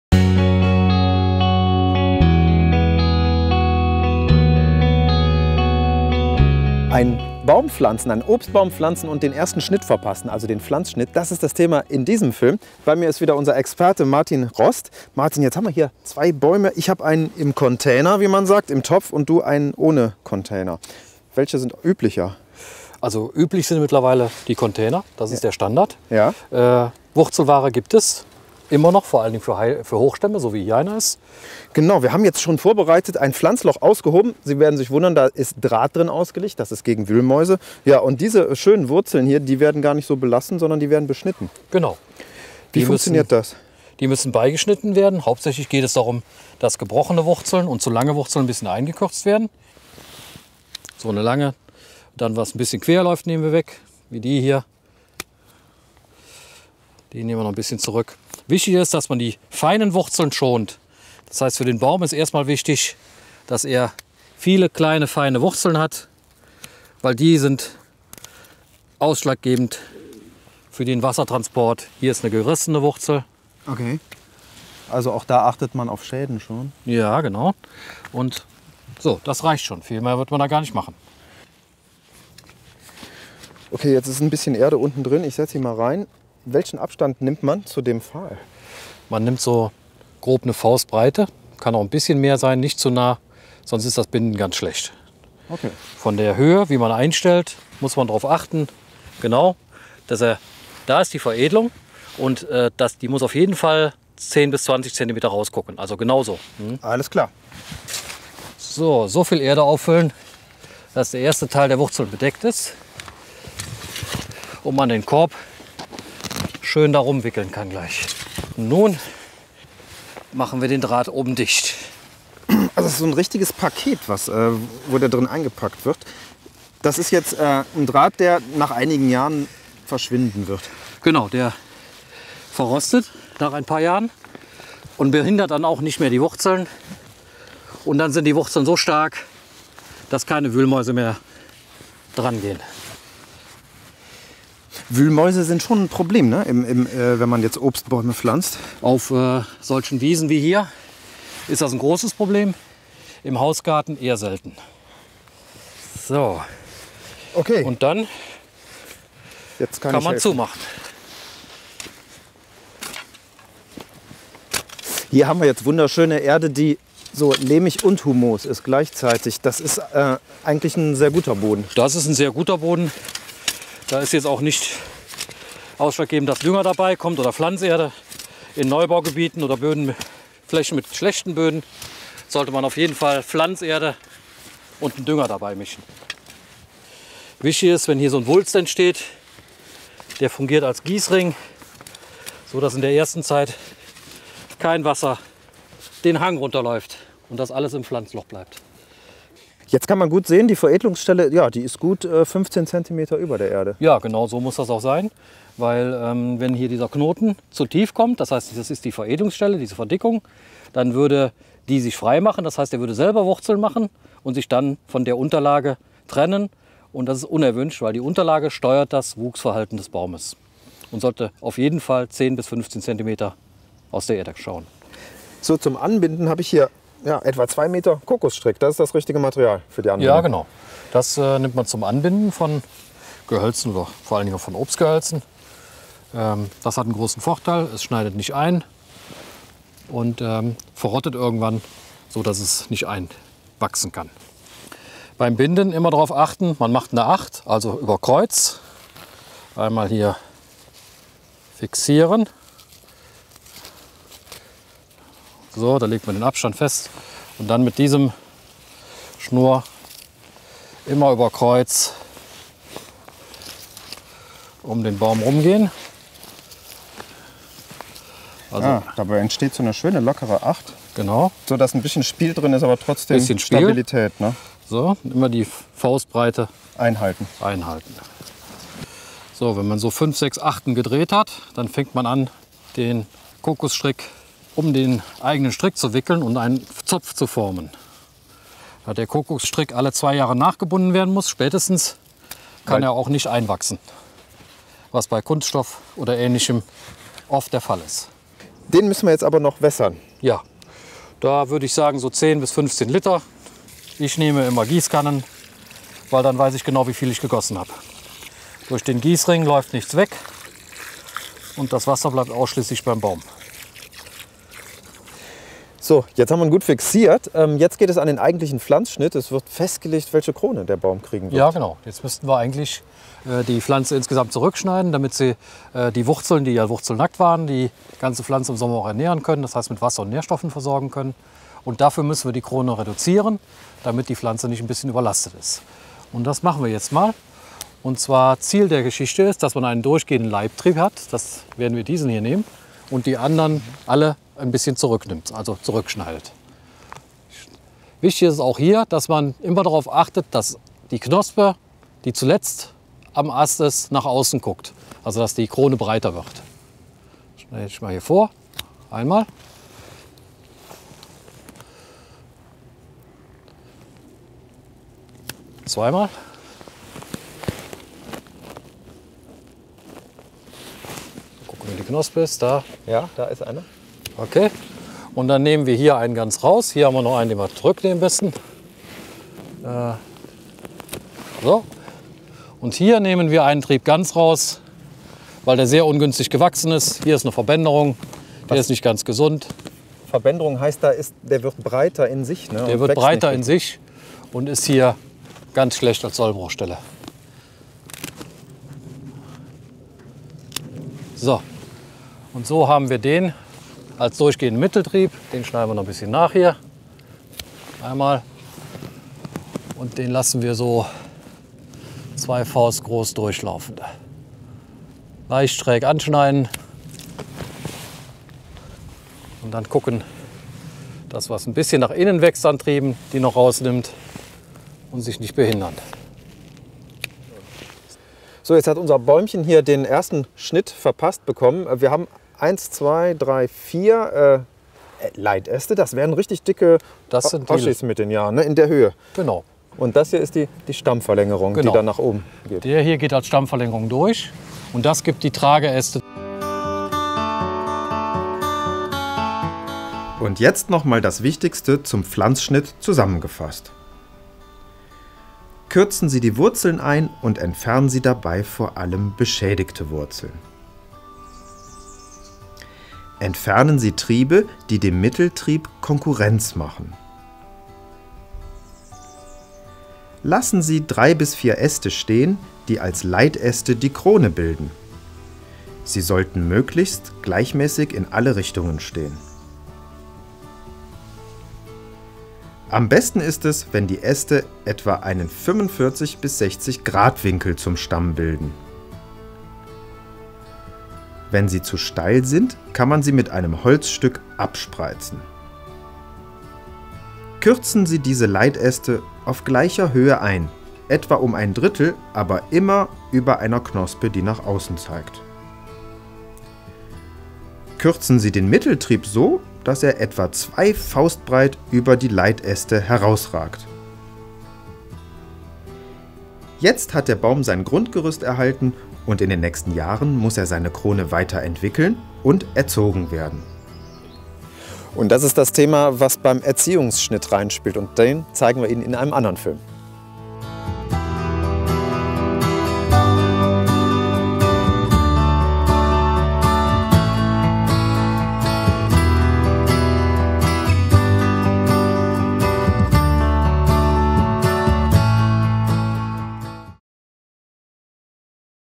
Obstbäume schneiden - Teil 2: Pflanzung und Pflanzschnitt
Der zweite Teil der Reihe "Obstbäume schneiden" zeigt, wie man einen Baum richtig pflanzt und den Pflanzschnitt ausführt.